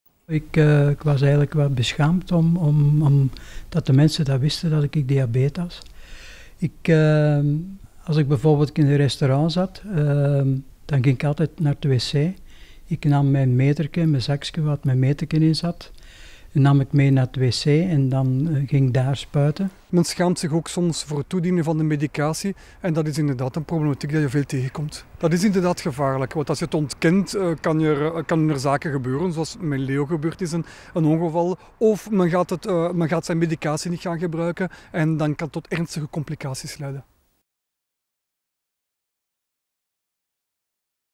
Wereld Diabetesdag: patiënten schamen zich vaak voor hun ziekte
In ons land hebben zo'n 400.000 mensen diabetes en elke 17 minuten krijgt in Vlaanderen iemand de diagnose van diabetes. En daarom wil de diabetesliga op deze Wereld Diabetesdag de ziekte in de schijnwerpers zetten. En vooral hoe belangrijk de preventie van diabetes wel is. Wij vonden in Turnhout iemand die al 20 jaar diabeet is en het daar vooral in het begin heel moeilijk mee had.